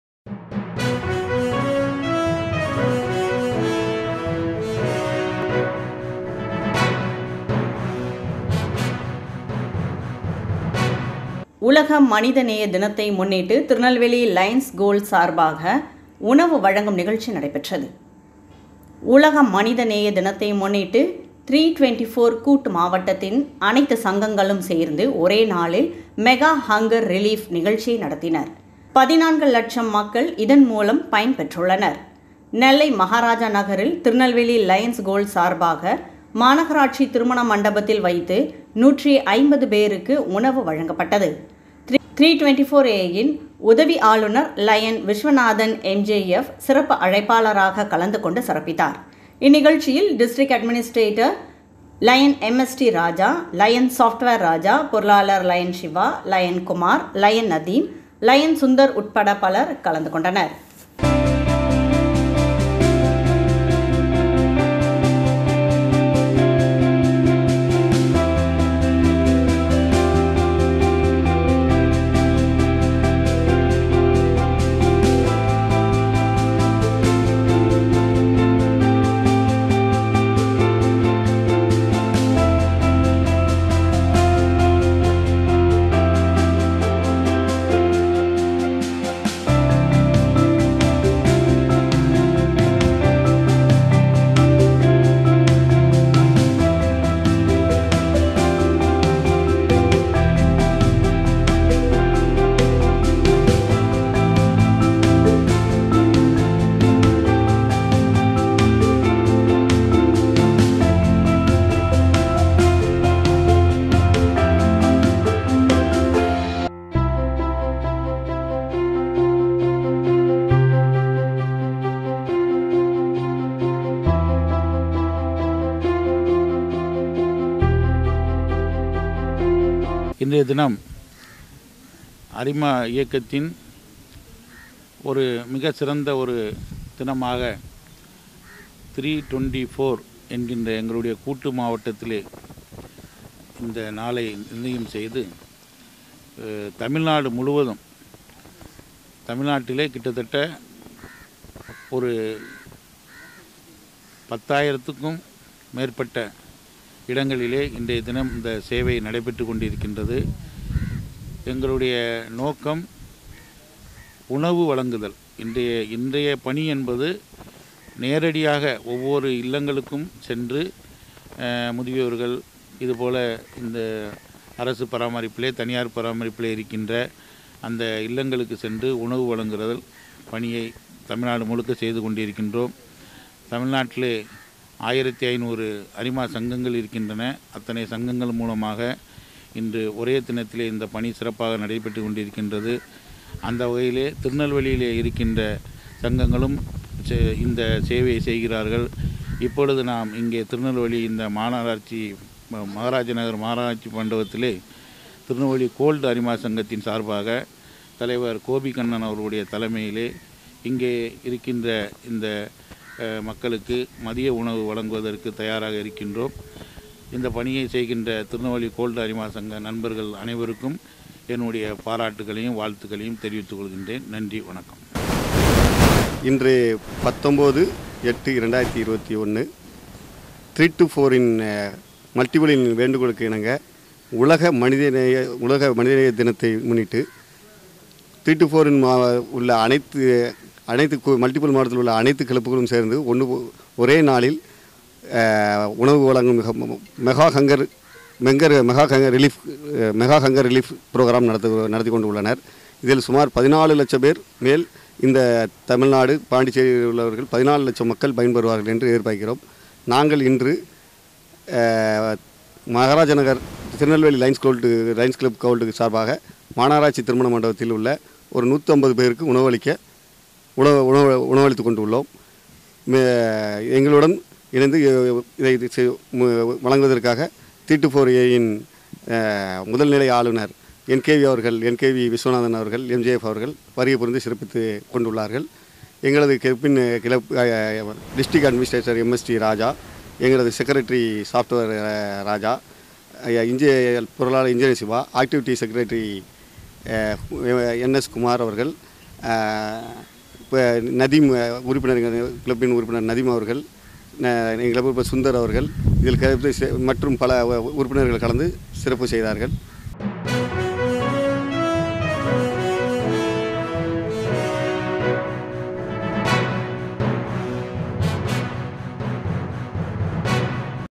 திருநெல்வேலி கோல்டு அரிமா சங்கம் சார்பில் உணவு வழங்கும் நிகழ்ச்சி | TAMIL | MAYURI TV
உலக மனிதநேய தினத்தை முன்னிட்டு திருநெல்வேலி கோல்டு அரிமா சங்கம் சார்பில் உணவு வழங்கும் நிகழ்ச்சி